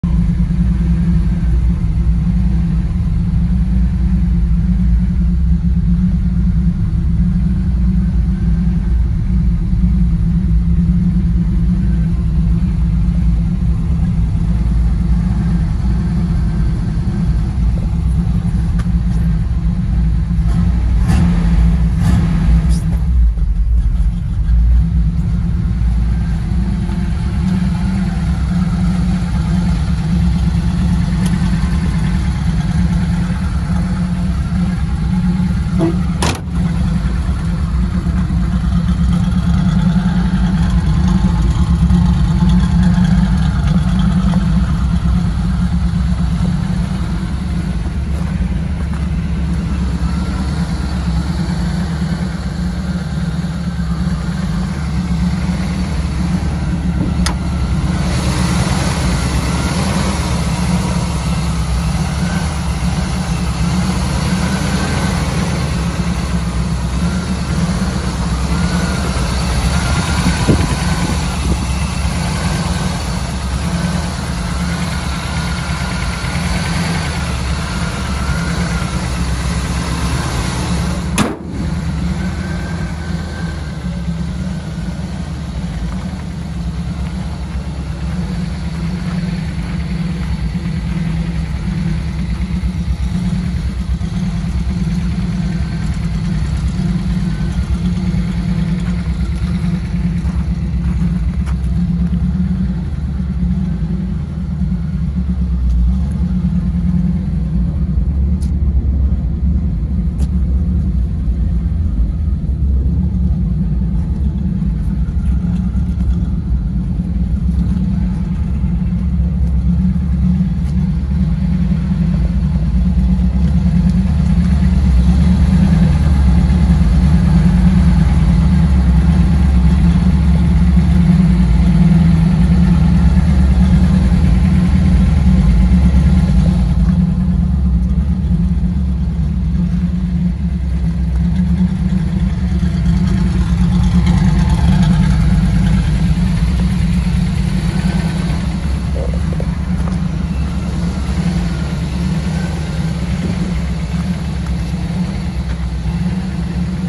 1970 “Hugger Orange” Chevrolet C10 Pickup “walk around” video
For sale!

Southern AL truck.
350ci V8